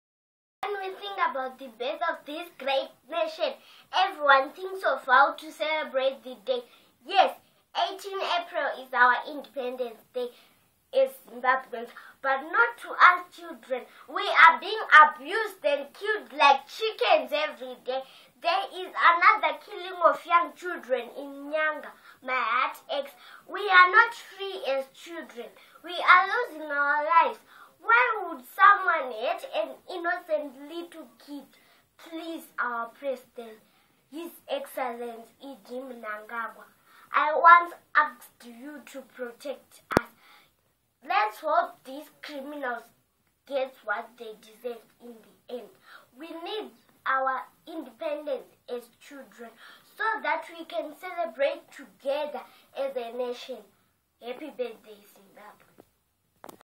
EIGHT-YEAR-OLD SPEAKS ON INDEPENDENCE!
AN EIGHT-YEAR-OLD Glen View 7 Primary School grade two pupil Micla Nokutenda Never recites her Independence poem. She has different poems ranging from different themes including Gender Based Violence and child abuse.